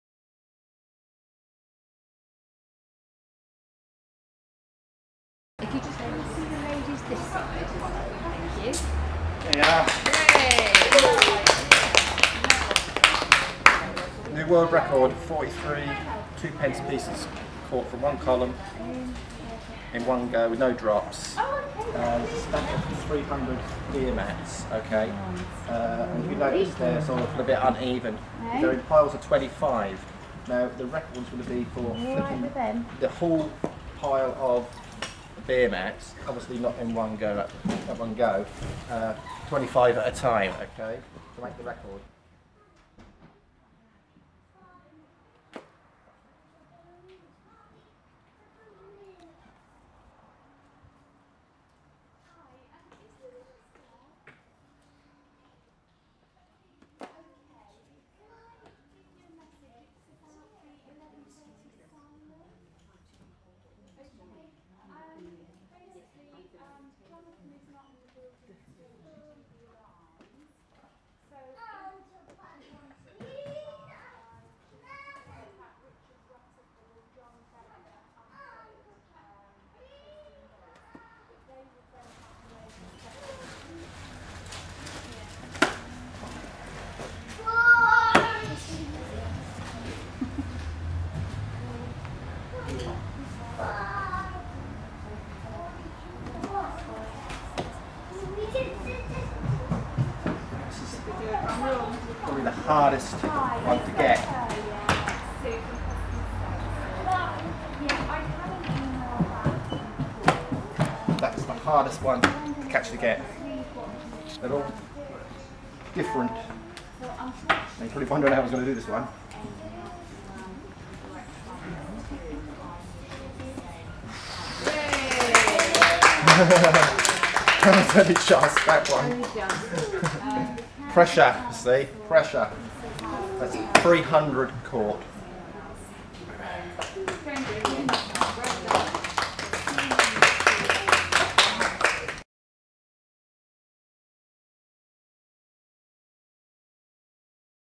Breaking records at Felixstowe Library
Felixstowe based record breaker Dean Gould broke two of his own records at Felixstowe Library to inspire pupils from Langer Primary Academy to get involved in this year's Summer Reading Challenge. The theme of the reading challenge is Record Breakers in 2015.